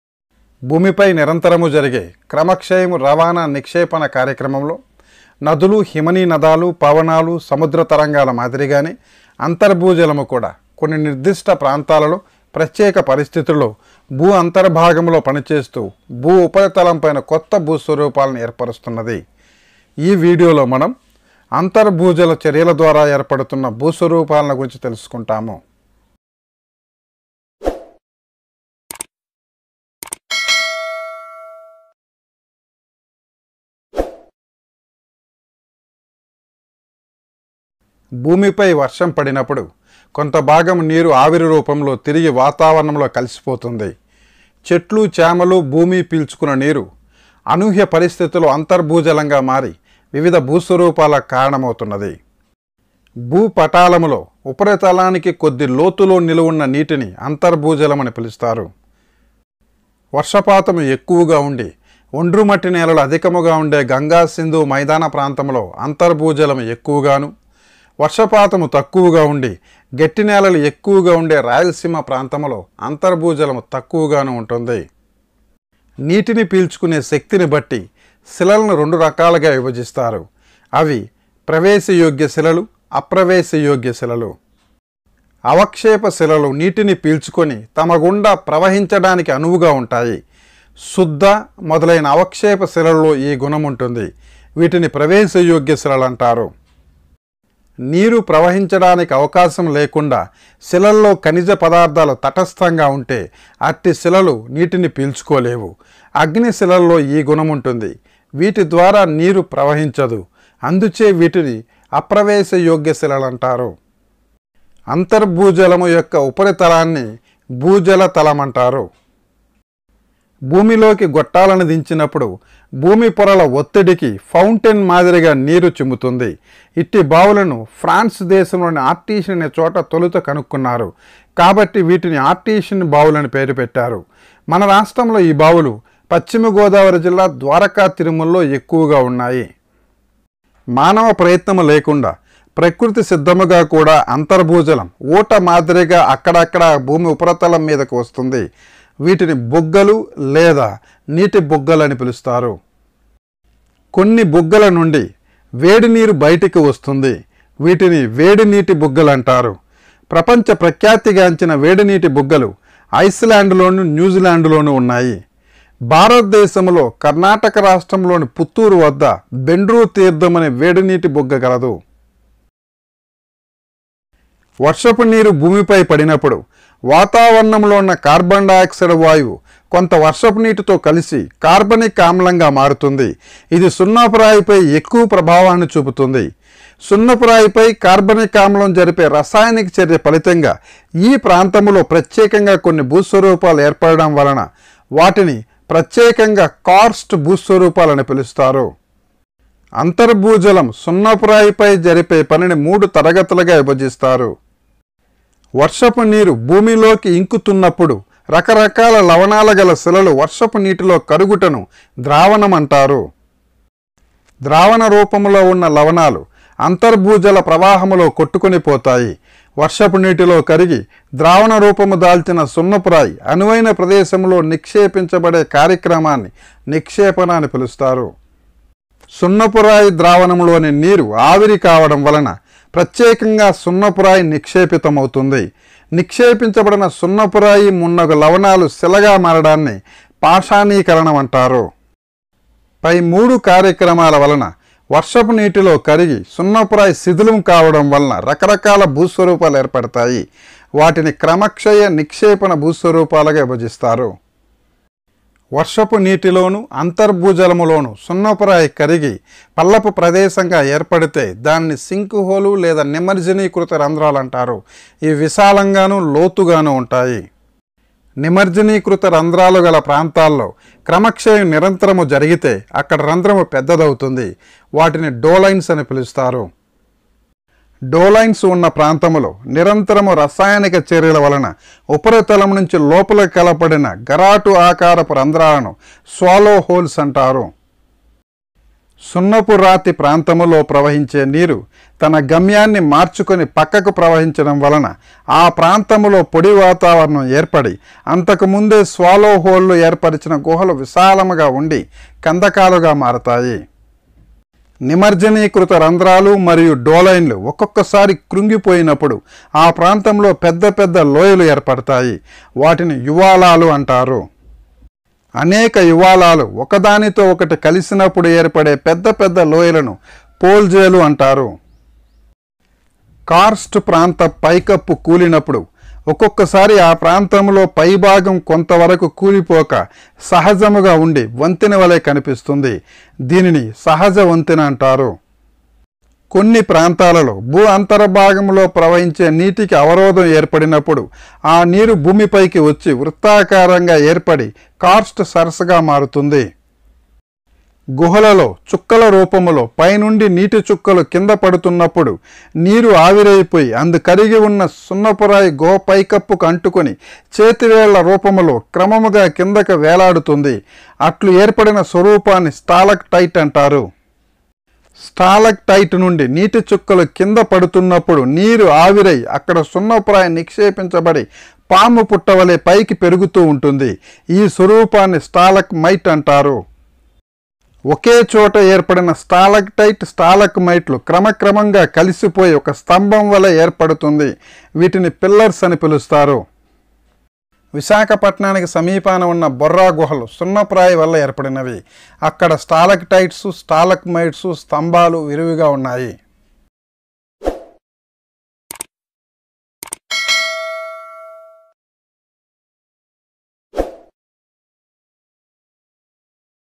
Karst topography(By Under groundwater)(అంతర్భూజల భూస్వరూపాలు)TET/DSC/All Compe Exams#Sreedhar Raju
భూమిపై నిరంతరం జరిగే క్రమక్షయ, రవాణా, నిక్షేపణ కార్యక్రమంలో నదులు, హిమనీనదాలు,పవనాలు,సముద్ర తరంగాల మాదిరిగానే అంతర్భూజలం కూడా కొన్ని నిర్దిష్ట ప్రాంతాల్లో, ప్రత్యేక పరిస్థితులలో భూఅంతర్భాగంలో కదులుతూ భూఉపరితలం పైన కొత్త భూస్వరూపాలను ఏర్పరుస్తున్నాయి.
ఈ వీడియో లో మనం అంతర్భూజల భూస్వరూపాల గురుంచి తెలుసుకుంటాము.